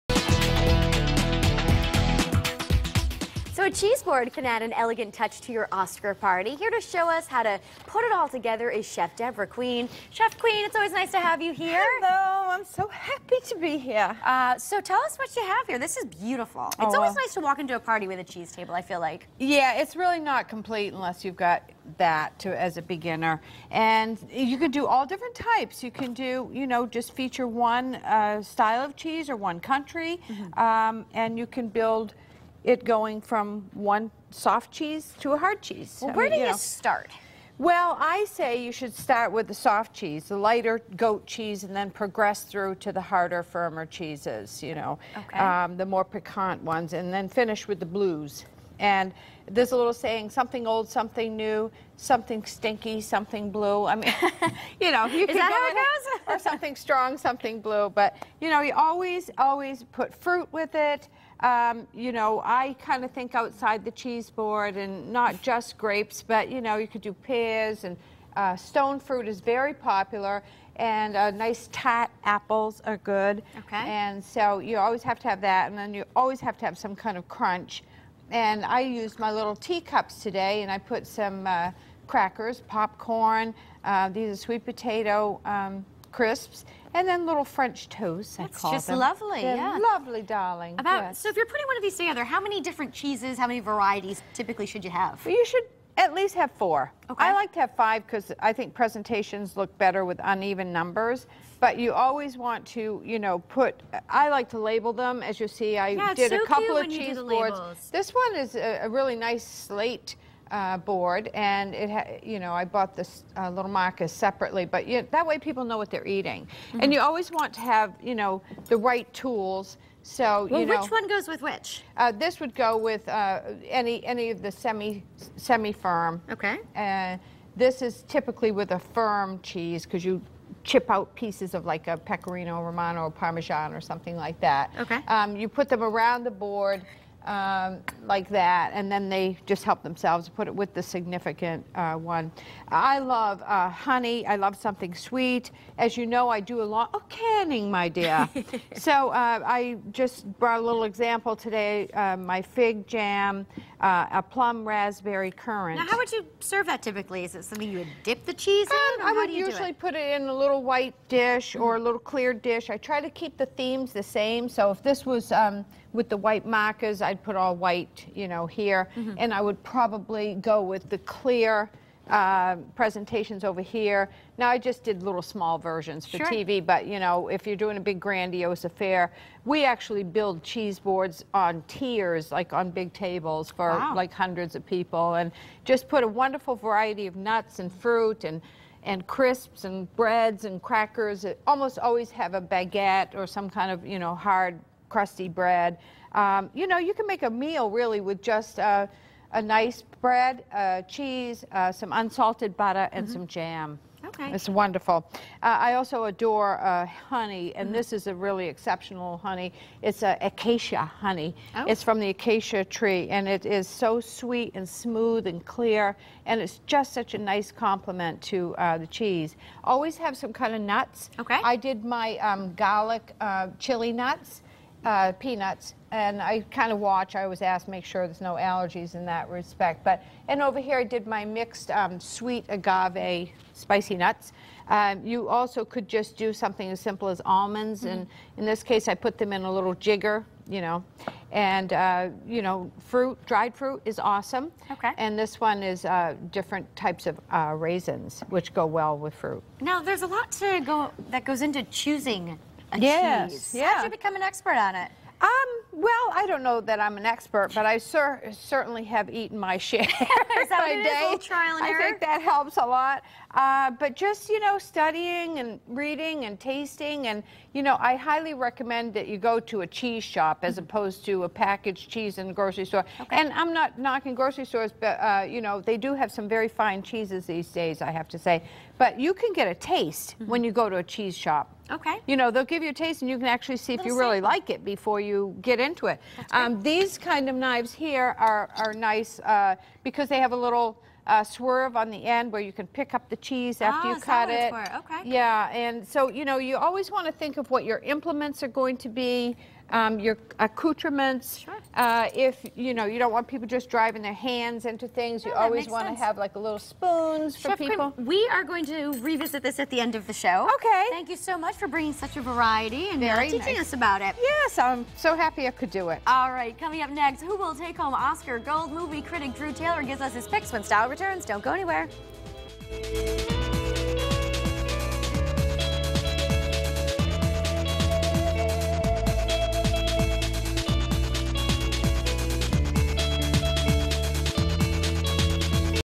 In the Kitchen: The Cheeseboard
Personal Chef and Friend of the Show, Debra Queen, was back in the kitchen showing Teresa how to put together a cheeseboard for her Oscar Night Party.